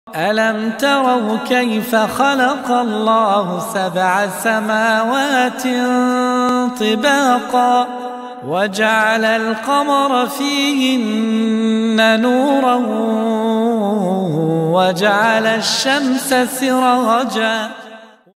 سورة نوح الاية 15 16
لقد استخدم نوح مع قومه أسلوباً علمياً في دعوته إلى الله تبارك وتعالى. فقد كان يأمرهم بالنظر إلى السماوات من فوقهم، ويأمرهم أن يتأمّلوا الشمس كيف تضيء وتبث الحرارة، ويأمرهم أن ينظروا إلى القمر كيف ينير في الليل. وهذا أسلوب علمي في الدعوة إلى الله فكيف استخدمه سيدنا نوح؟ لنقرأ هذه الآيات من سورة نوح، وفيها يخاطب نوح قومه: (أَلَمْ تَرَوْا كَيْفَ خَلَقَ اللَّهُ سَبْعَ سَمَاوَاتٍ طِبَاقًا * وَجَعَلَ الْقَمَرَ فِيهِنَّ نُورًا وَجَعَلَ الشَّمْسَ سِرَاجًا) [نوح: 15-16].